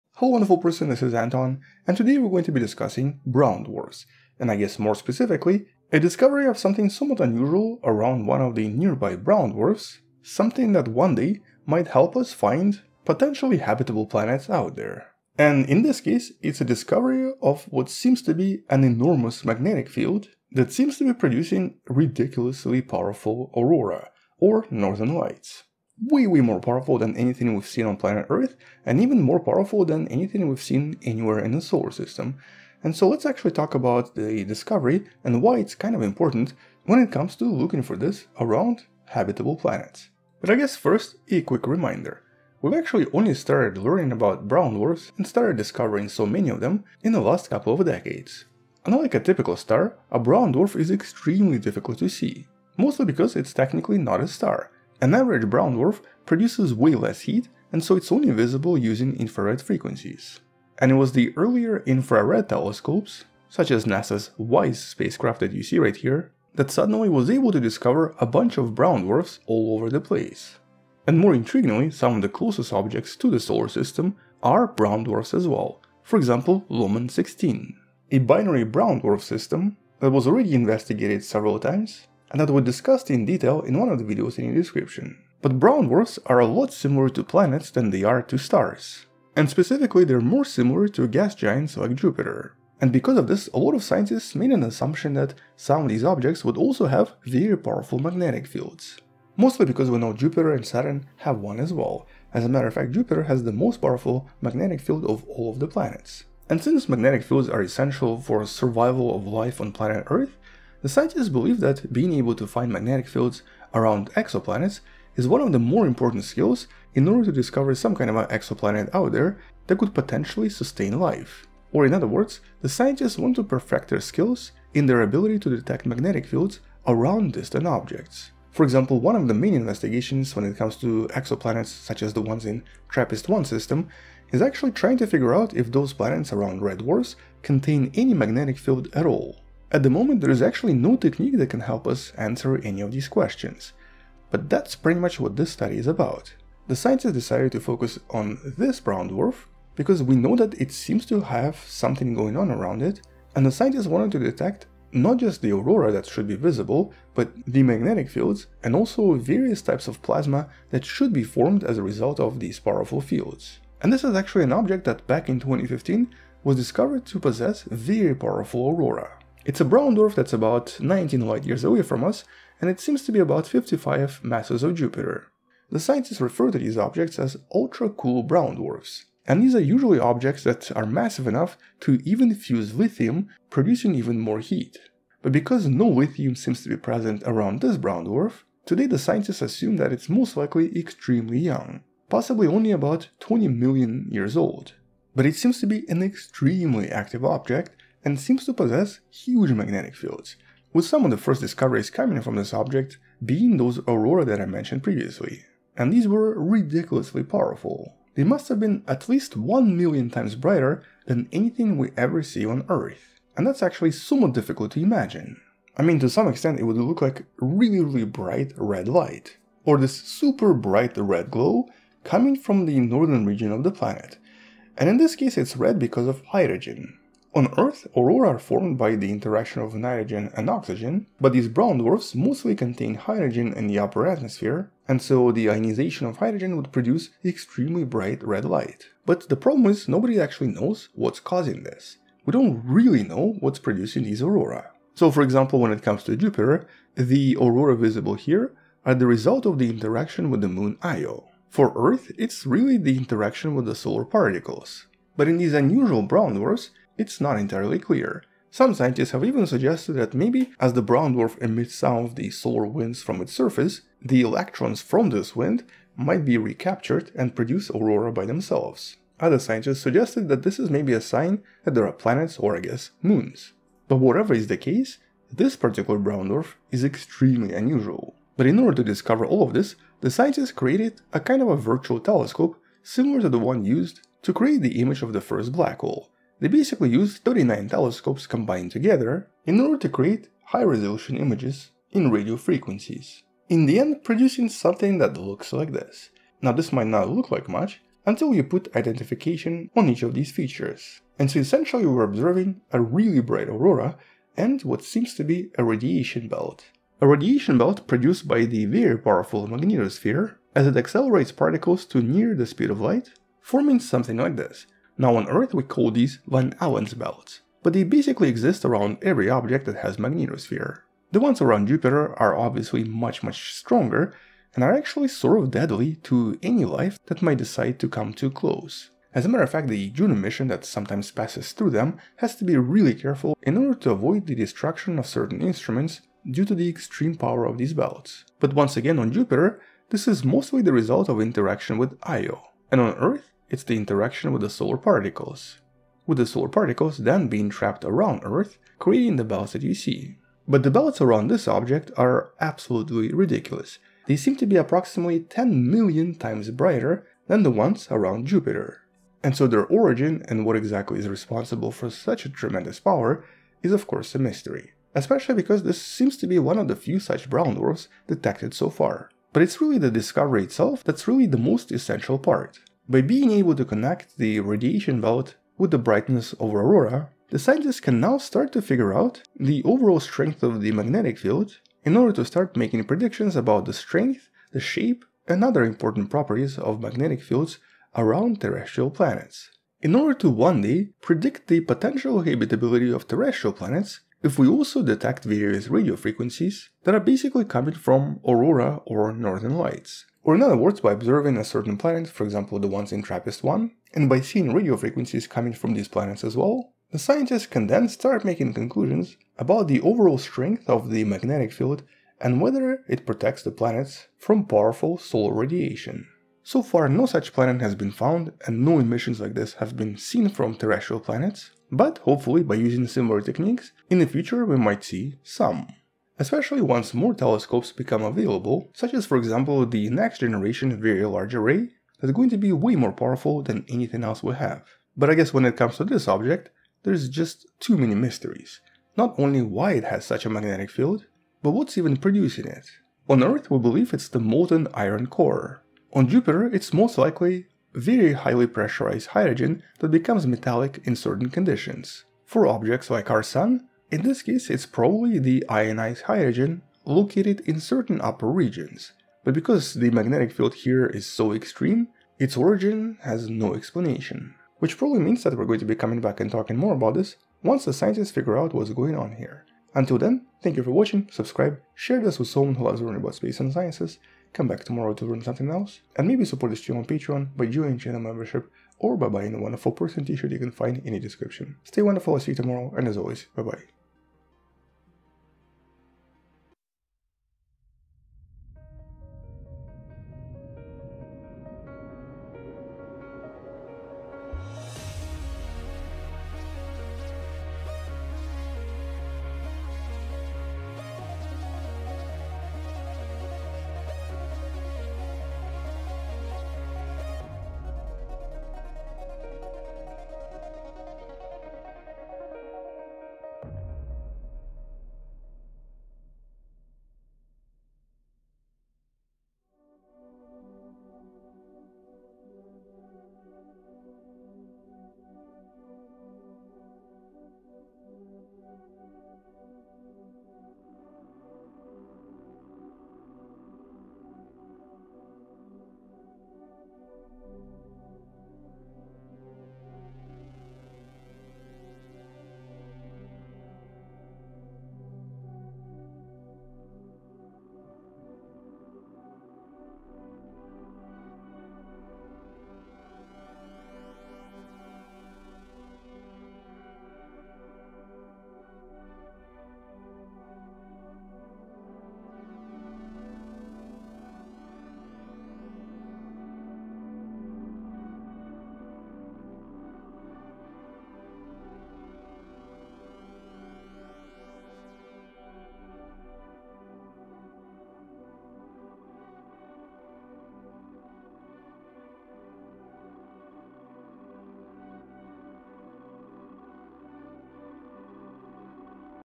Brown Dwarf With Unbelievably Powerful Magnetosphere, 1000000x Jupiter
More cool designs are on Amazon

Hello and welcome! My name is Anton and in this video, we will talk about discoveries from a nearby brown dwarf

0:00 Intro to the new discovery
0:40 What we know about brown dwarfs so far
2:15 What the new study is all about
2:40 Strange brown dwarf in the study
4:30 How scientists discovered new features
5:40 Ridiculous power of this brown dwarf
7:00 Unexplained questions

ASTRON/Danielle Futselaar